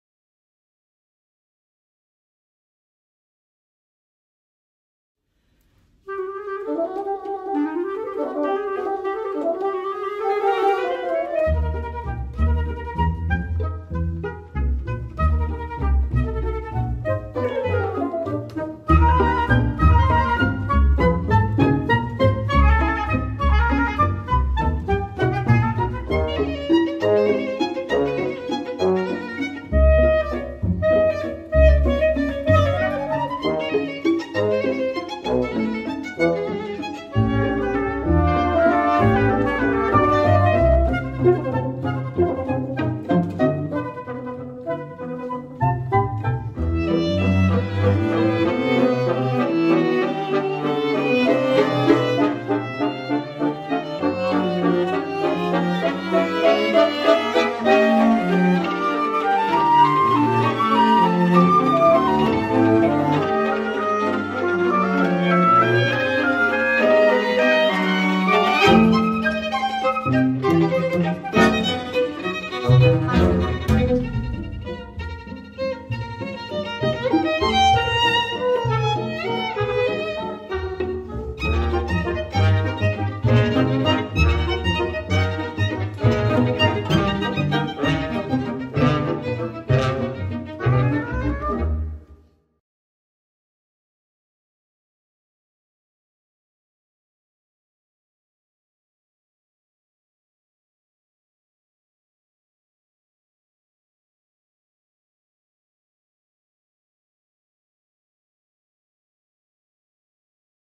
DeBezettingSpeelt Jean Françaix
Live registratie uit 2010, van een concert in WorldArtcentreDelft